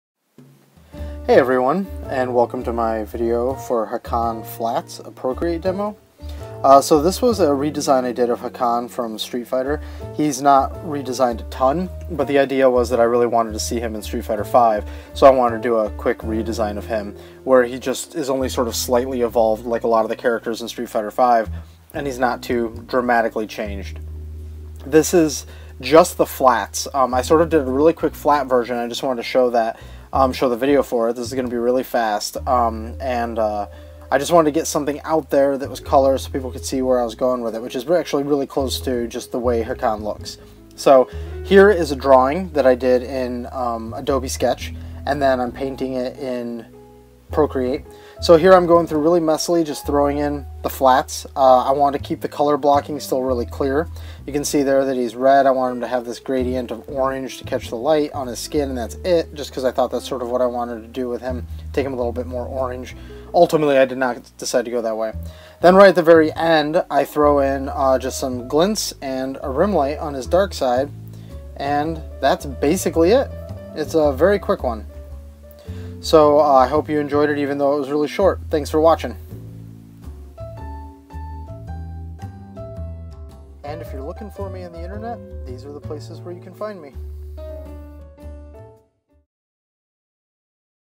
Ep.13| Hakan Flats - Procreate Demo
Quick video on the flats for my Street Fighter 5 redesign of Hakan.